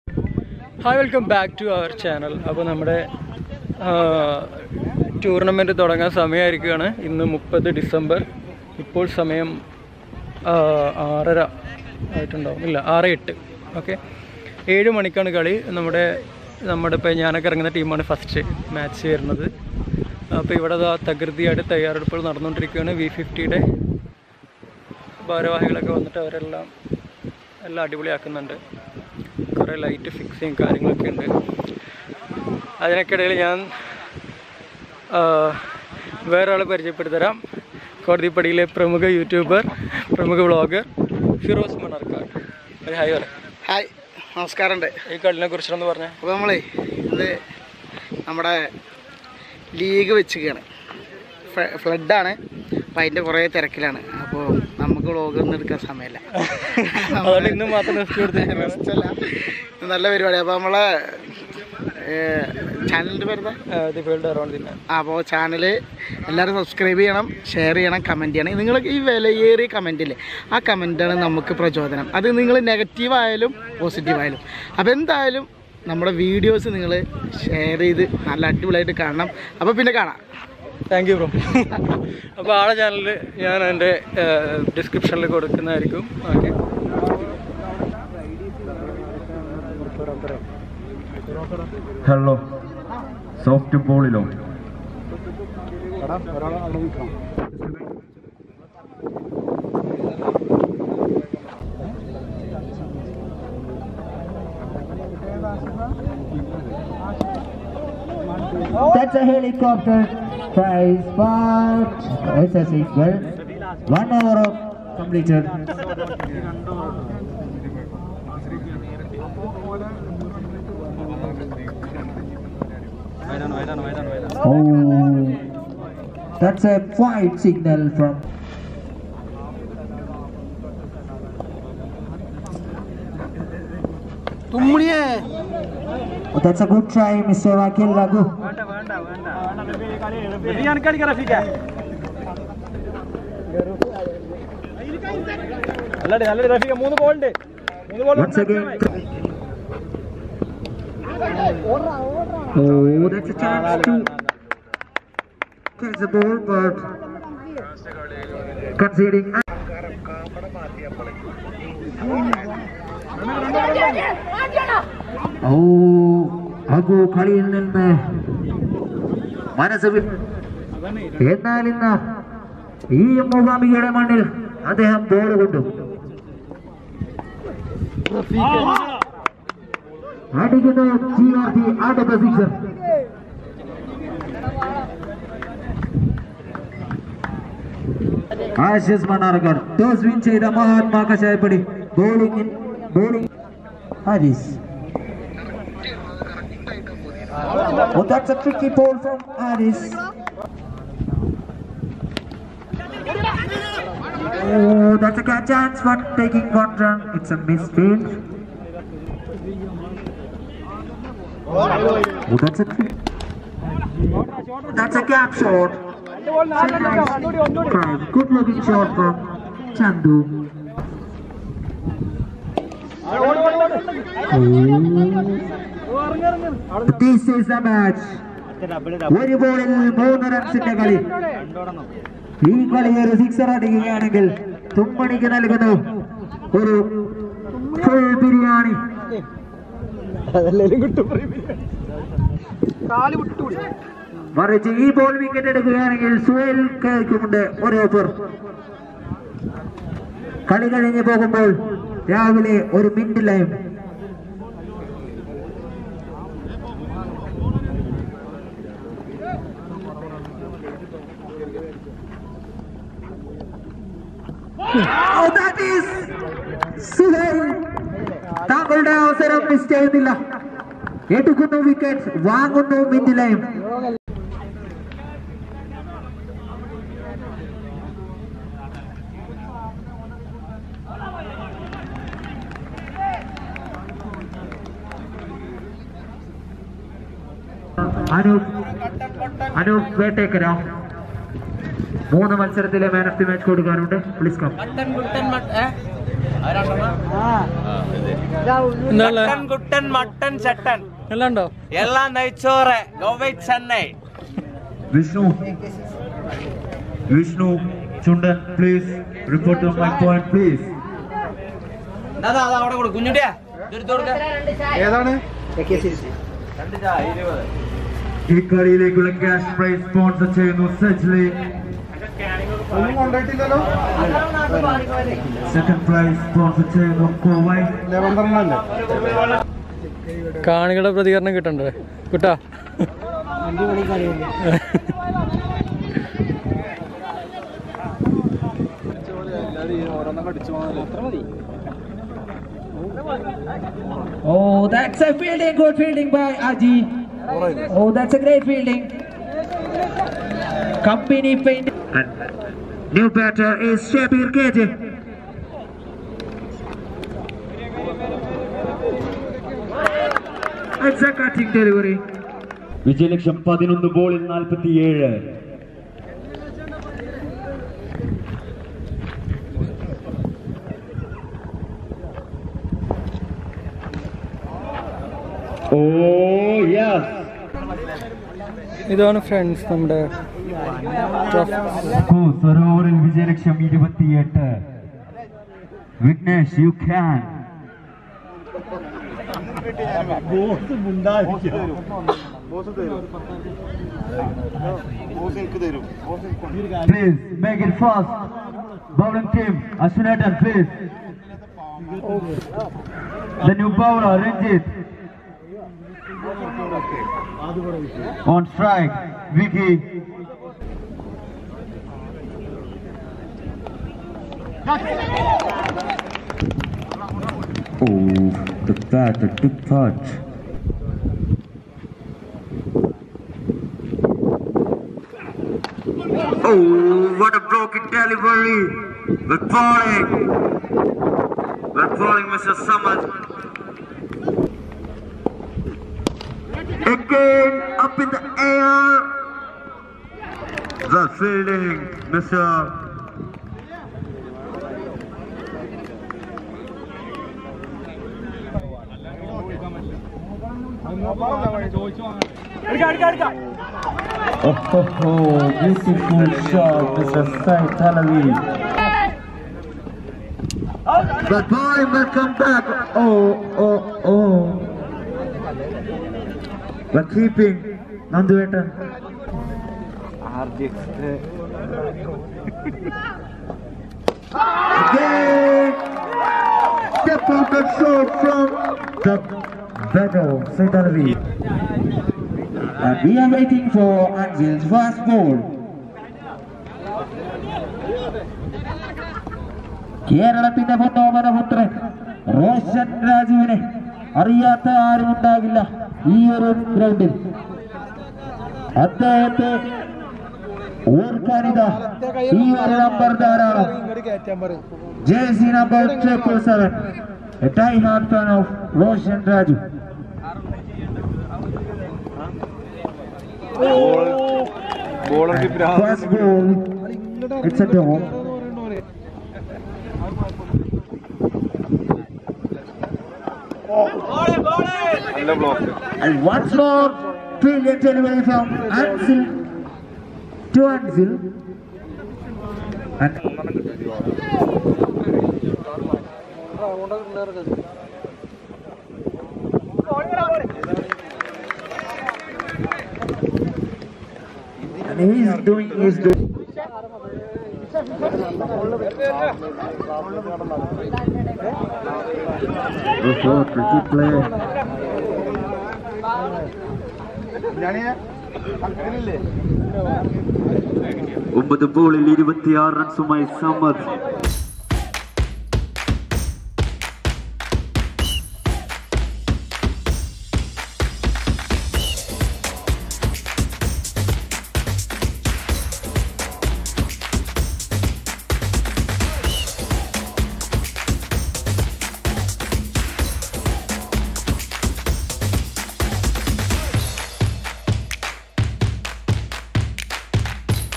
Softball Cricket Tournament 2023 | WE-50 Arts & Sports Club | Mannarkkad
The cricket tournament is scheduled for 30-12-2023 at Mookambika School Ground, Mannarkkad. There are 12 teams grouped into 3 groups. Each group has 11 players.
Group A : KCC Mannarkkad, Go Ahead Mannarkkad, AKCC Vadasserippuram & Future Spot Sreekrishnapuram
Group B : WE-50 Kodathippadi, Ashes Mannarkkad, Mahathma Kashayappadi & NKH
Group C : One Touch Mannarkkad, Boss & Boys, Rebel Riders Mannarkkad & Komban Elevens

Firos Mannarkkad Channel Link: @firosmkd5468